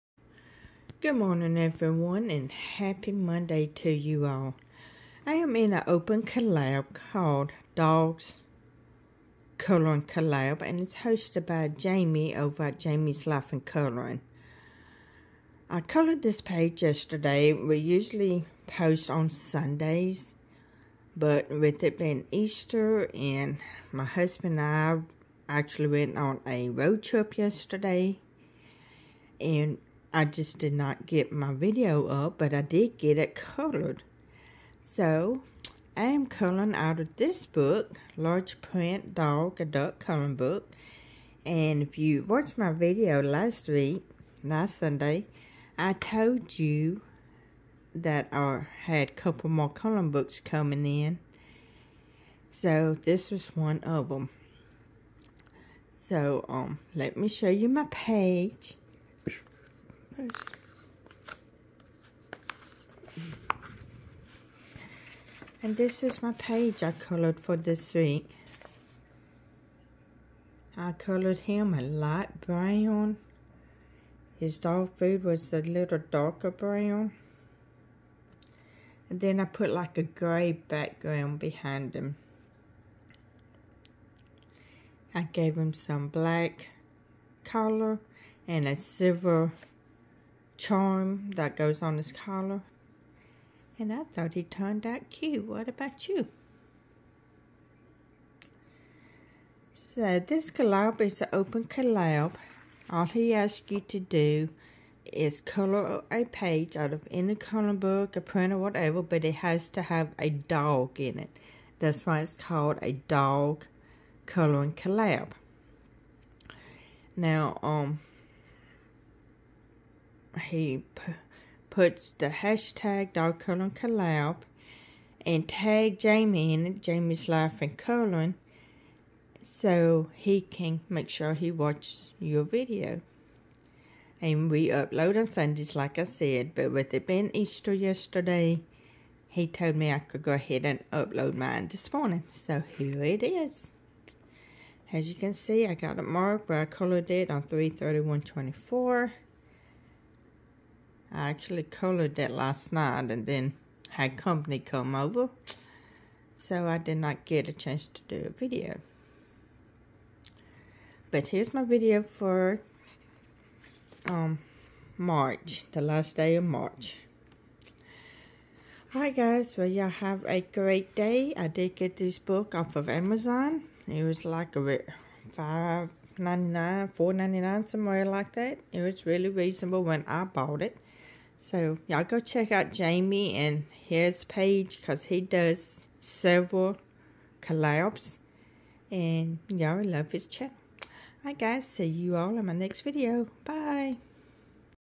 dogscoloringcollab @Disneyandcoloringwithjamie 3.31.24/4.1.24
In this video, I participated in the dog coloring collab hosted by Jamie from Jamie's Life and Coloring. The hashtag and the link yo Jamie's channel are in the title of this video. Go check both links out.

Friend Mail: Address upon request